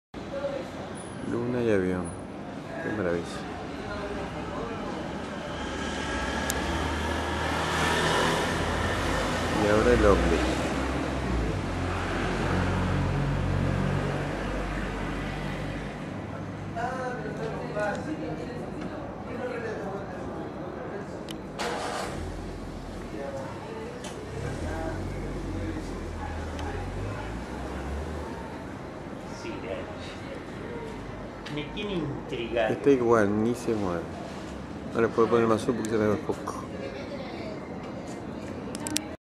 Unidentified Flying Object (UFO) in PARANA ARGENTINA! 12/06/2013
A neighbor alerted us! We looked at the sky and saw it.

Filmed from neighborhood Villa Sarmiento, Paraná, Entre Rios, in the home garden.